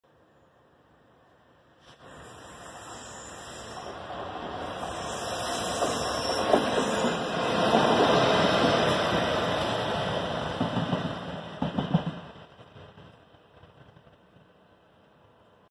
DBC 66142 "Maritime Intermodal Three" passing Chertsey on route learning duties on 15/12/22
DBC 66142 "Maritime Intermodal Three" passing Chertsey on route learning duties on 15/12/22 - 0Z51 09:14 Hither Green Loco Sidings to Woking